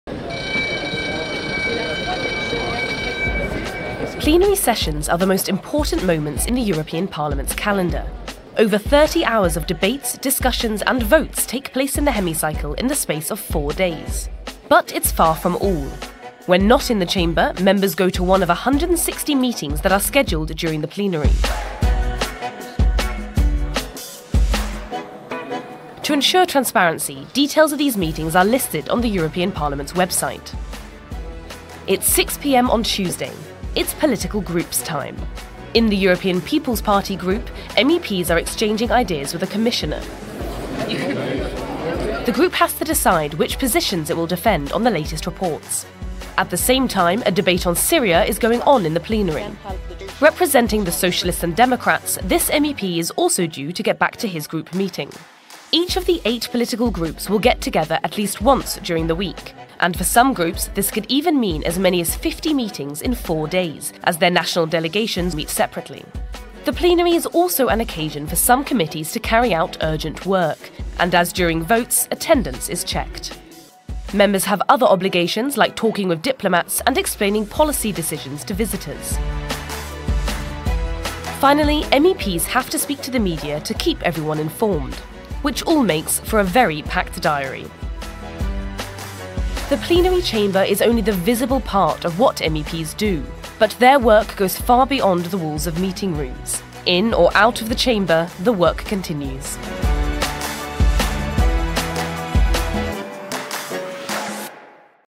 Backstage of a plenary
Behind the scenes of the plenary sessions of the European Parliament, MEPs have a lot to do: political group meetings, coordinator meetings, committee meetings, delegation meetings, press conferences and interviews. The reason is simple: when they’re not sitting in the hemicycle, they have to prepare the votes and the positions they want to defend.